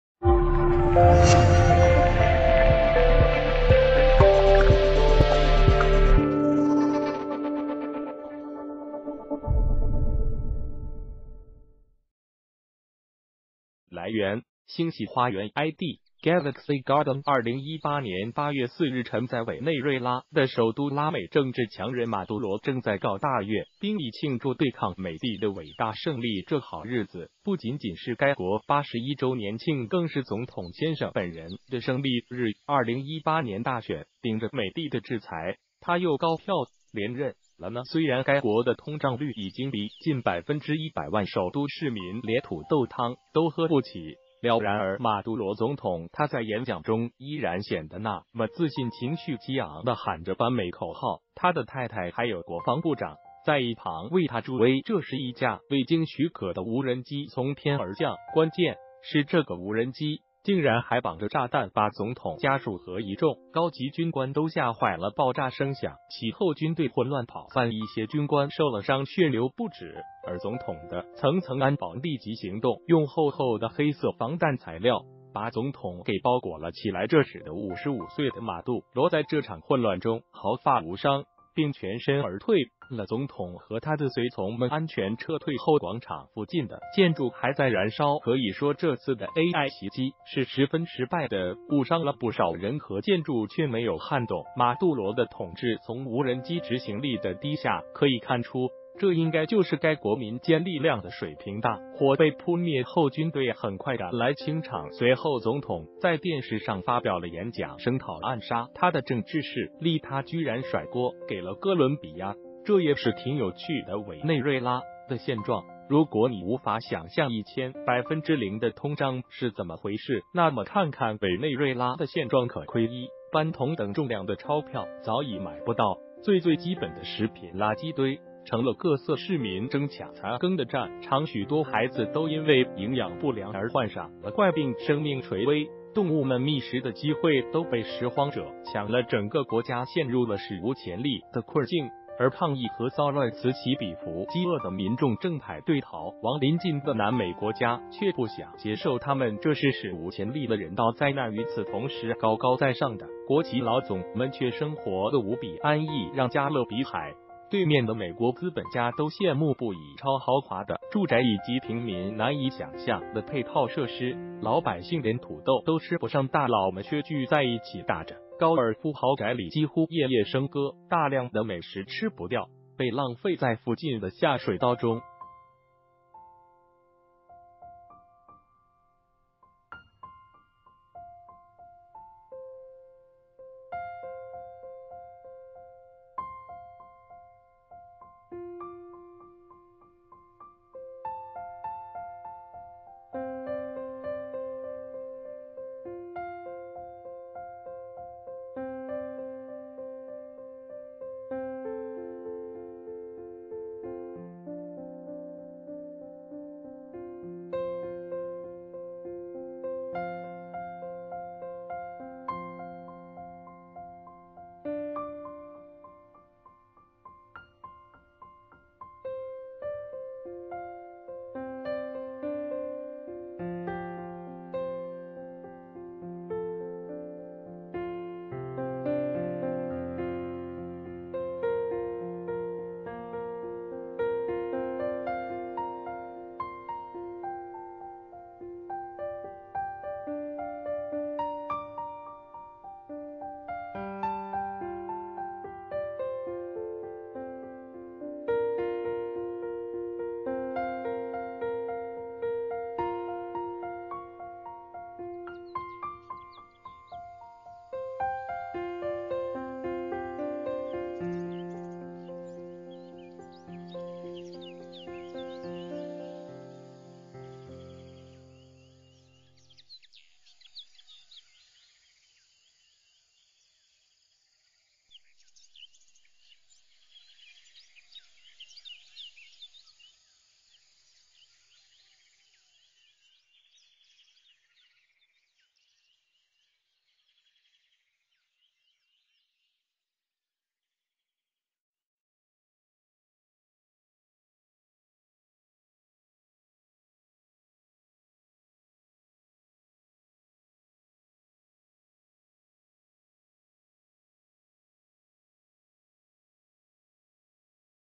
总统正在激情澎湃的演讲，突然飞来了一架无人机
谢谢！来源：星系花园ID：Galaxy_garden2018年8月4日晨在委内瑞拉的首都拉美政治强人马杜罗正在搞大阅兵以庆祝对抗美帝的伟大胜利这好日子不仅仅是该国81周年庆更是总统先生本人的胜利日2018年大选顶着美帝的制裁他又高票连任了呢虽然该国的通胀率已经逼近1000000%首都市民连土豆汤都喝不起了然而马杜罗总统他在演讲中依然显得那么自信情绪激昂地喊着反美口号他的太太还有国防部长在一旁为他助威这时一架未经许可的无人机从天而降关键是这个无人机竟然还绑着炸弹把总统家属和一众高级军官都吓坏了爆炸声响起后军队混乱跑散一些军官受了伤血流不止而总统的层层安保立即行动用厚厚的黑色防弹材料把总统给包裹了起来这使得55岁的马杜罗在这场混乱中毫发无伤并全身而退了总统和他的随从们安全撤退后广场附近的建筑还在燃烧可以说这次的AI袭击是十分失败的误伤了不少人和建筑却没有撼动马杜罗的统治从无人机执行力的低下可以看出这应该就是该国民间力量的水平大火被扑灭后军队很快赶来清场随后总统在电视上发表了演讲声讨暗杀他的政治势力他居然甩锅给了哥伦比亚这也是挺有趣的委内瑞拉的现状如果你无法想象1000000%的通胀是怎么回事那么看看委内瑞拉的现状可窥一斑同等重量的钞票早已买不到最最基本的食品垃圾堆成了各色市民争抢残羹的战场许多孩子都因为营养不良而患上了怪病生命垂危动物们觅食的机会都被拾荒者抢了整个国家陷入了史无前例的困境而抗议和骚乱此起彼伏饥饿的民众正排队逃亡临近的南美国家却不想接受他们这是史无前例的人道灾难与此同时高高在上的国企老总们却生活得无比安逸让加勒比海对面的美国资本家都羡慕不已超豪华的住宅以及平民难以想象的配套设施老百姓连土豆都吃不上大佬们却聚在一起打着高尔夫豪宅里几乎夜夜笙歌大量的美食吃不掉被浪费在附近的下水道中